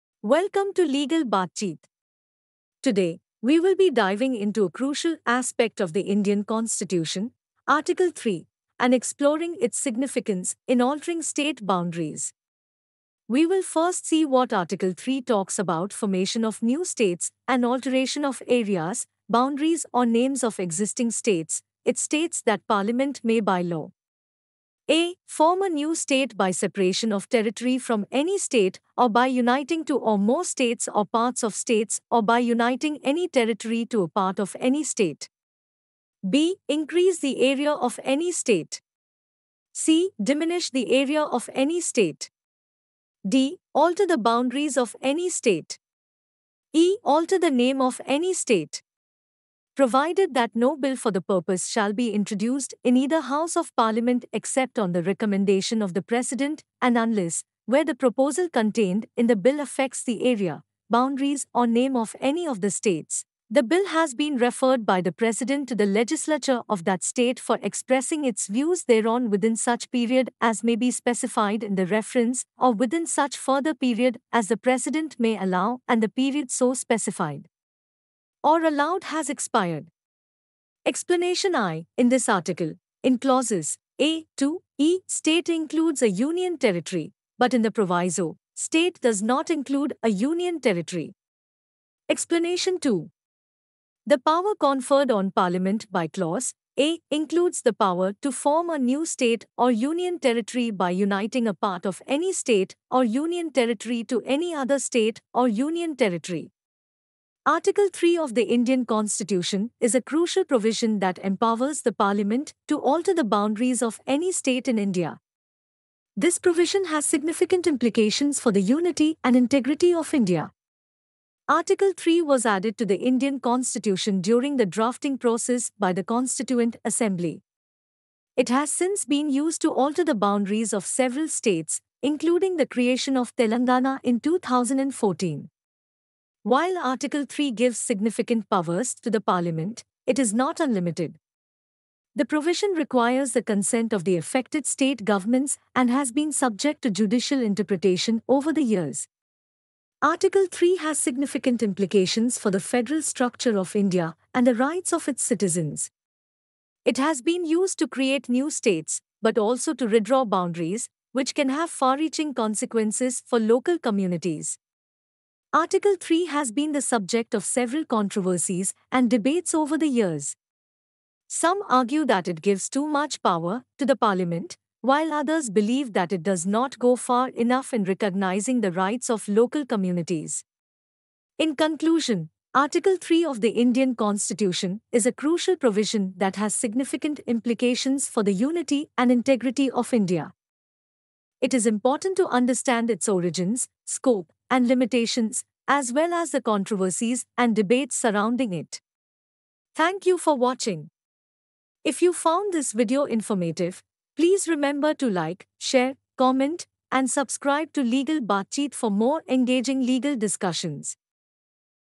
Demystifying Article 3 of Indian Constitution: Power of Altering State Boundaries
In this engaging video, join us as we dive deep into Article 3 of the Indian Constitution, exploring its significance in altering state boundaries. Discover the origins, evolution, and scope of this crucial provision, along with its implications for the federal structure of India and the rights of its citizens. We'll delve into the controversies and debates surrounding Article 3, providing a comprehensive understanding of its impact. Don't miss out on this informative discussion!